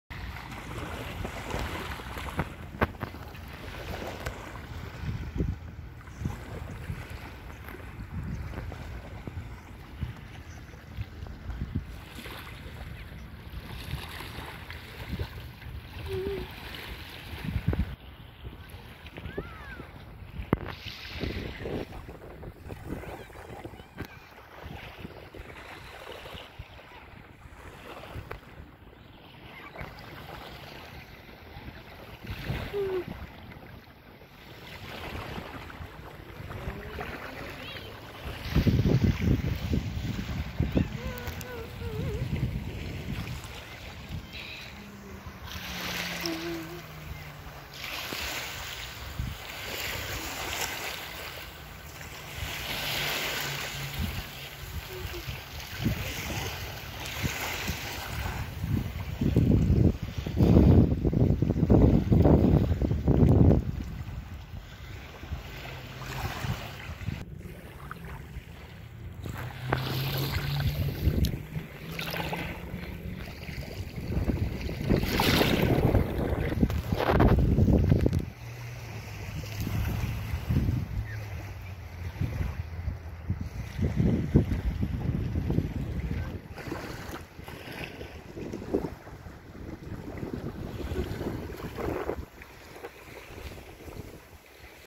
Port Melbourne Beach on a sunny day 21/9/20
I went to Port Melbourne Beach on a sunny day

People you must sub to

THANKS FOR MAKING IT TO THE END OF THE